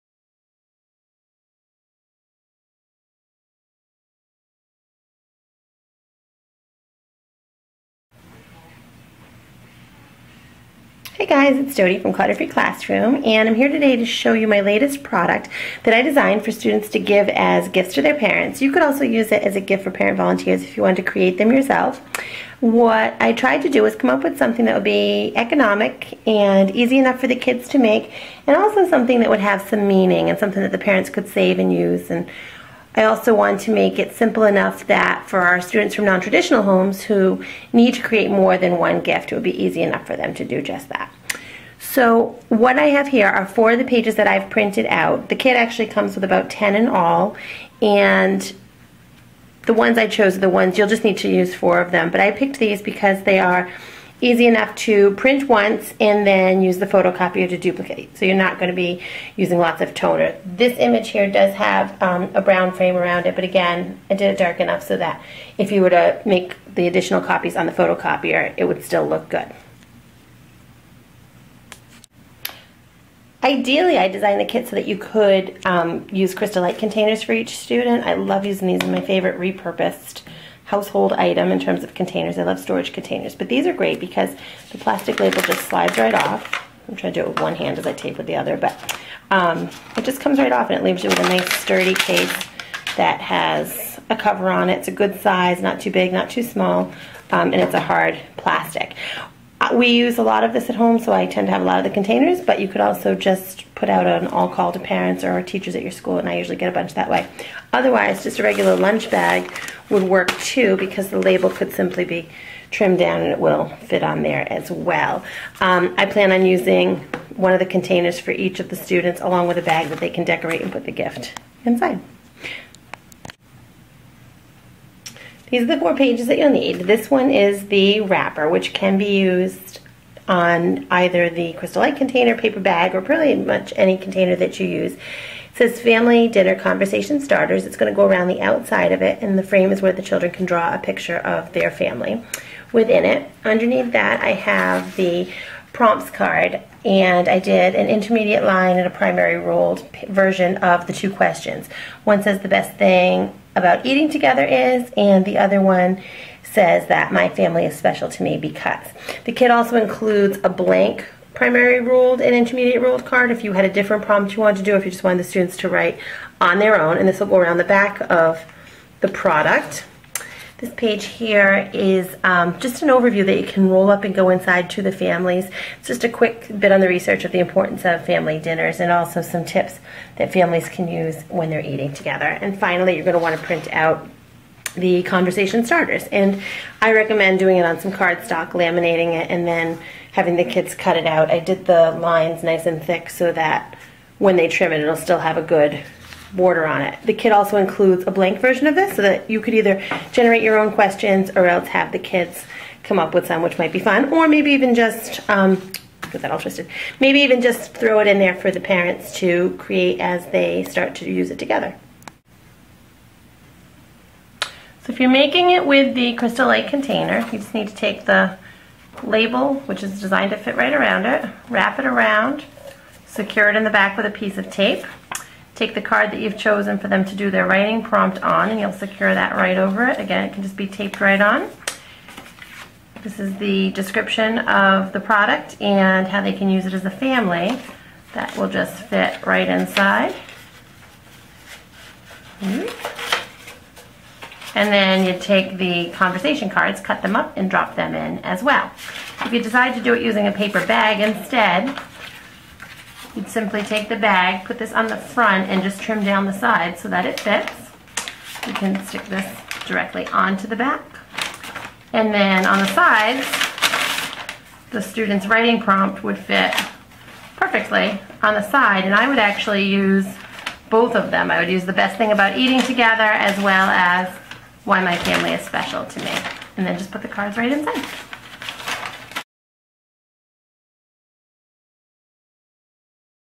Dinner Table Conversation Starters
This product is designed to be a gift for students to make and give their families for the holidays.  It would also make a nice end-of-year present.  I've been using it with my own family and LOVE how it has inspired conversations at our table and really kept us in tune with each other.